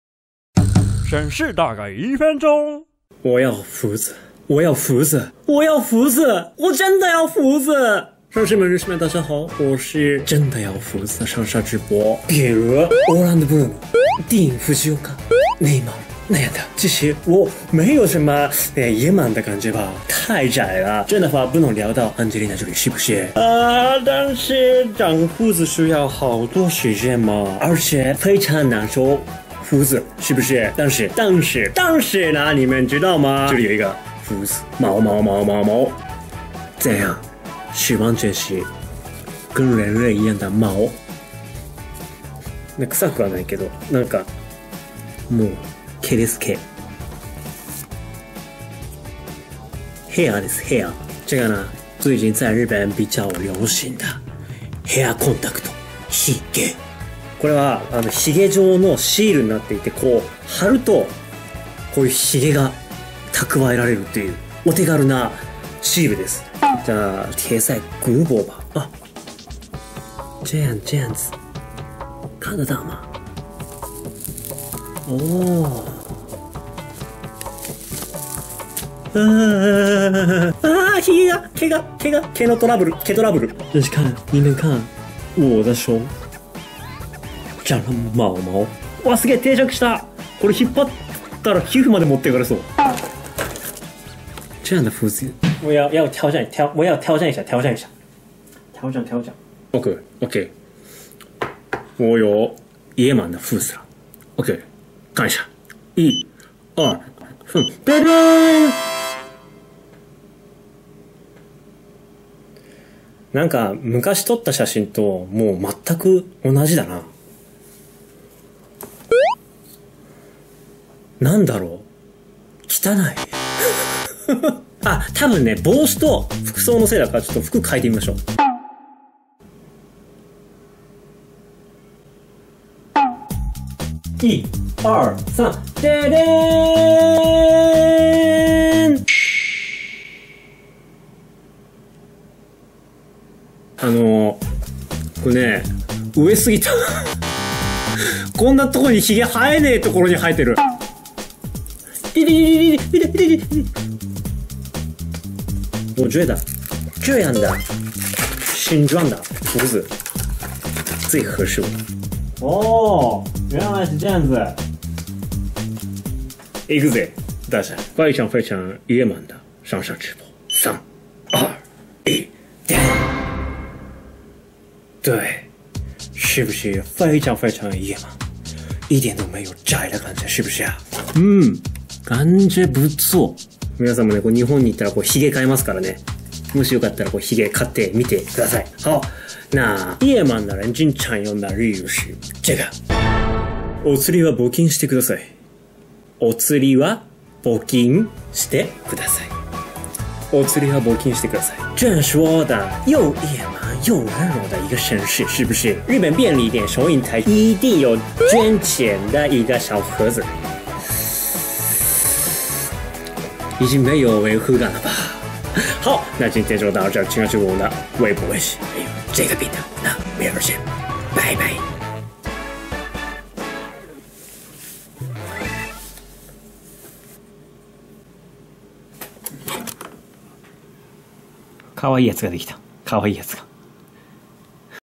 裝了日本假鬍子結果...！【紹介日本製產品】
不够男人？不够野性？山下33告诉你如何做一个真正的男人